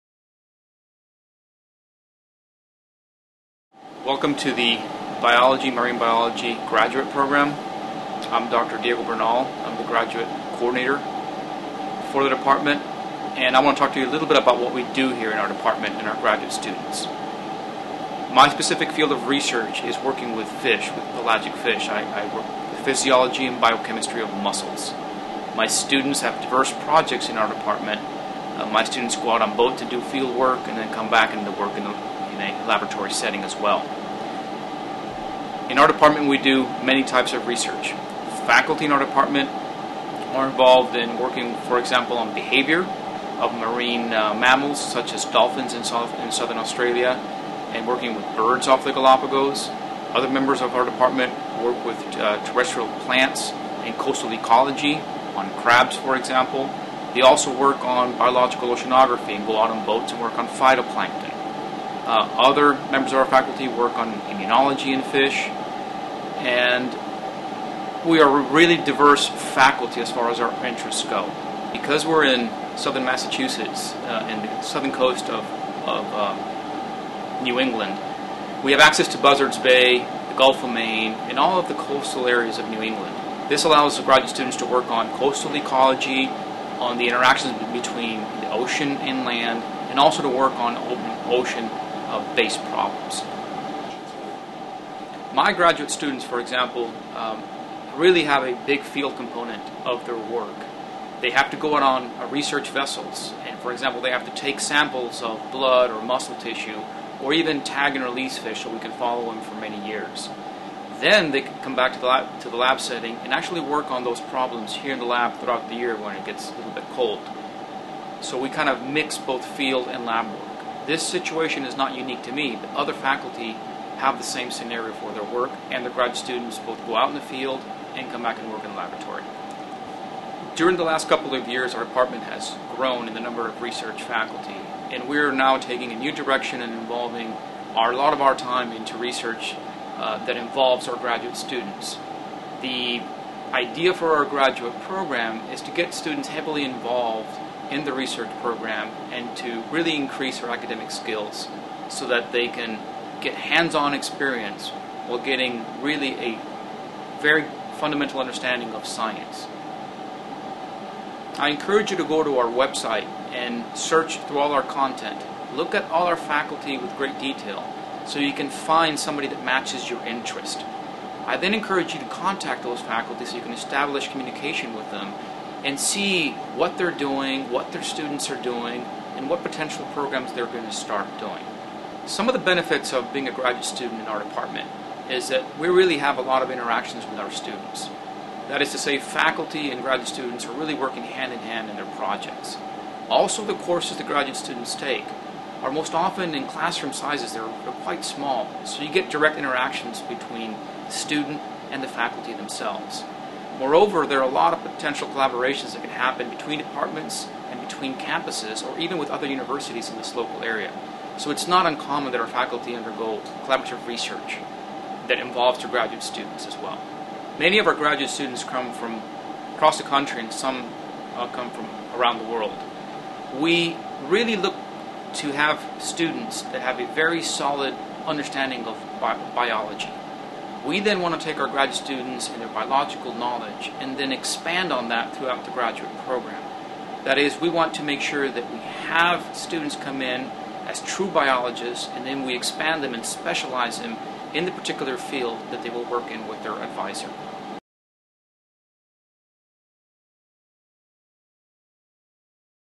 Welcome to the Biology Graduate Program
Biology Graduate Program Director
Diego Bernal